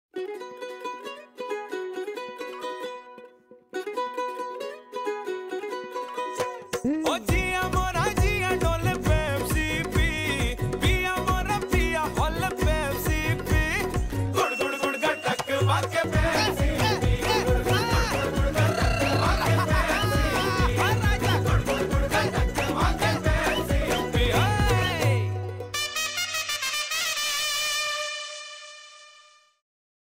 Crash the Pepsi IPL-Finalist-India Celebrates Pepsi IPL
Eight teams. One country. One drink to bring them all together . Watch it abhi, Shiva Rana's Pepsi ad. You can help choose the final wining ad. Voting is on from May 8-May 14